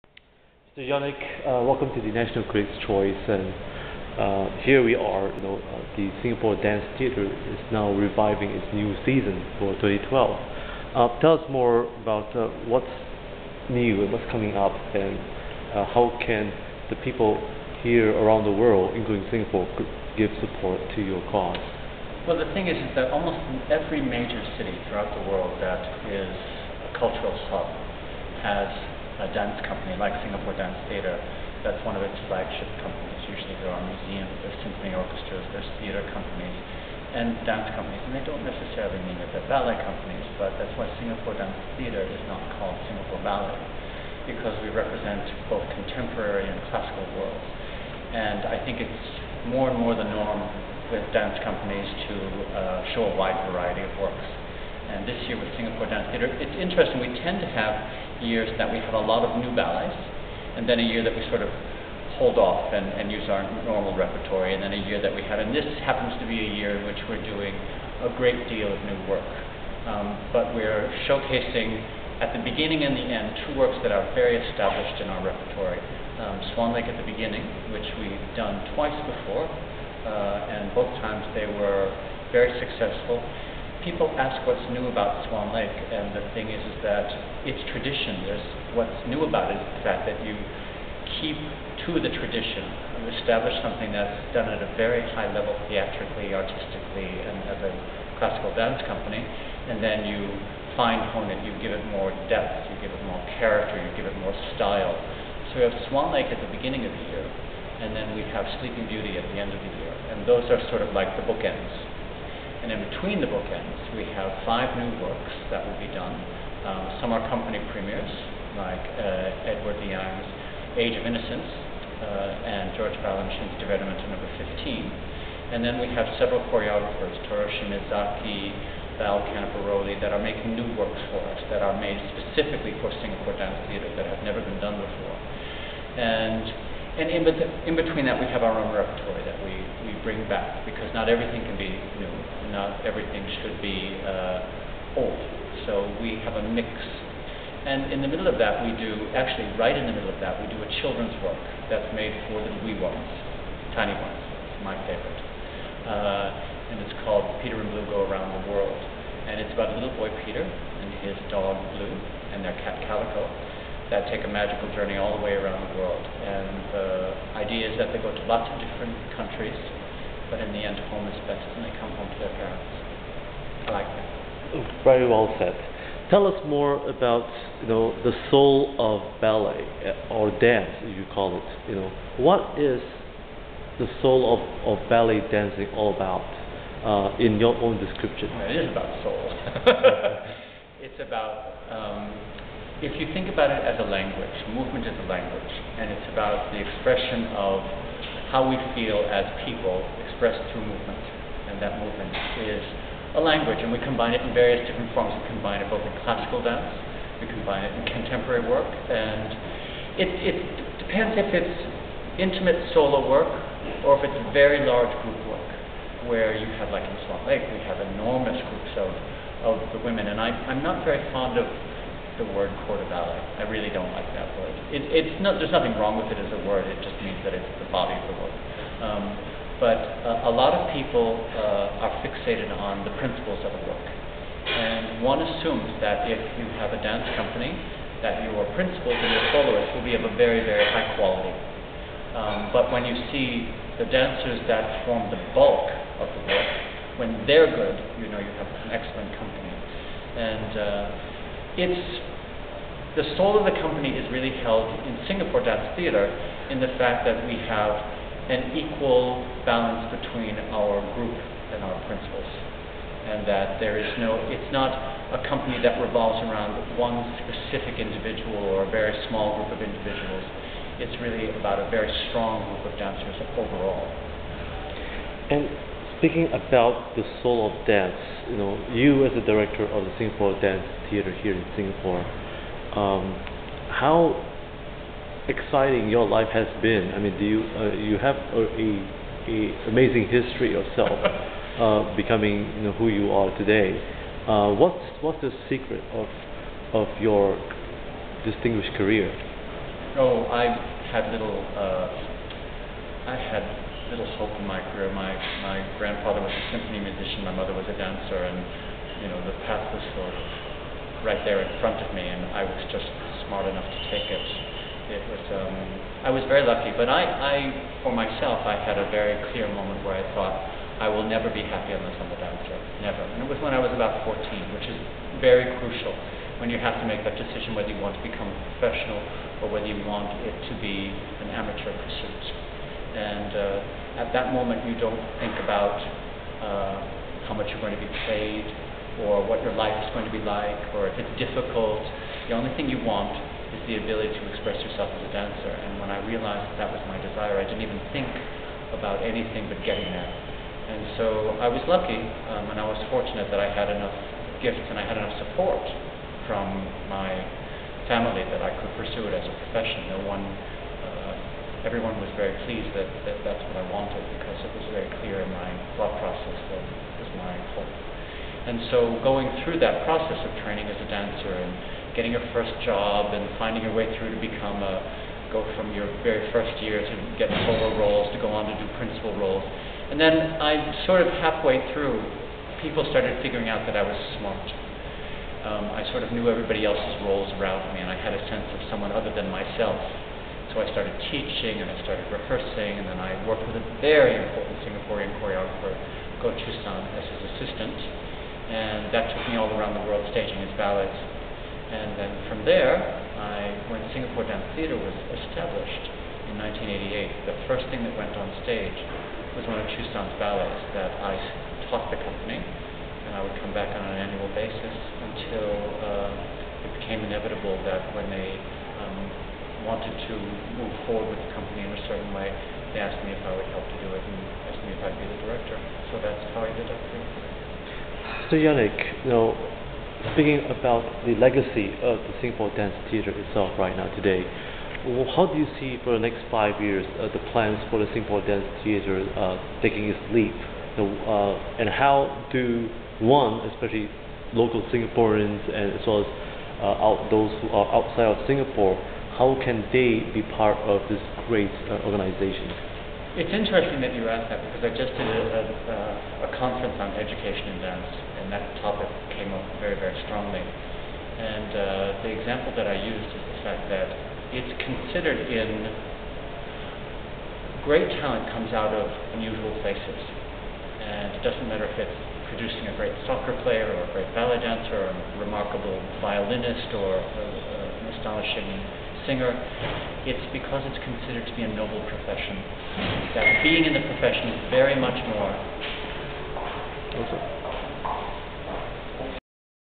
Face to Face with Janek Schergen with Robin Stienberg, National Critics Choice Part 1
Face to Face with Janek Schergen with Robin Stienberg, Editor in Chief of the National Critics Choice Online News was held on January 31st 2012. Janek Schergen is the Artistic Director of the Singapore Dance Theatre, Asia's most renowned Dance organization's in the world.